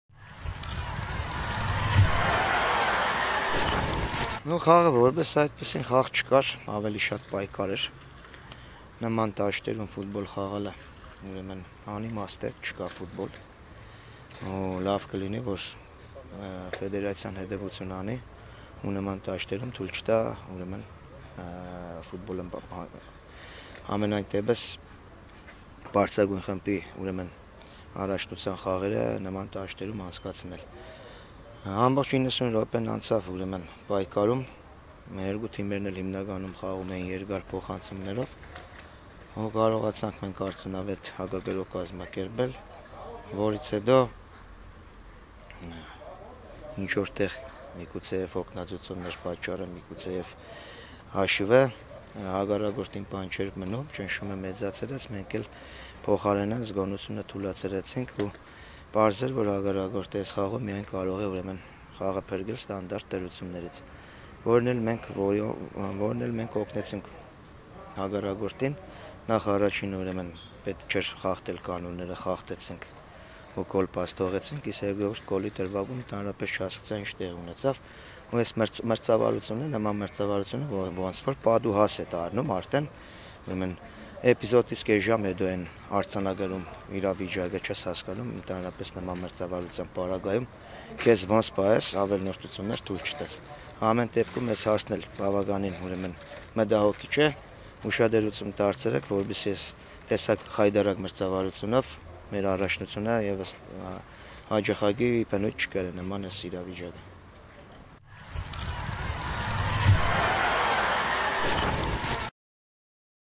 Vardan Bichakhchyan about the Armenian PL Matchday 3 match vs FC Noah (11.09.20)
Վարդան Բիչախչյանը VBET Հայաստանի Պրեմիեր լիգայի 3-րդ տուրի Նոա ՖԱ դեմ կայացած խաղի մասին (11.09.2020)։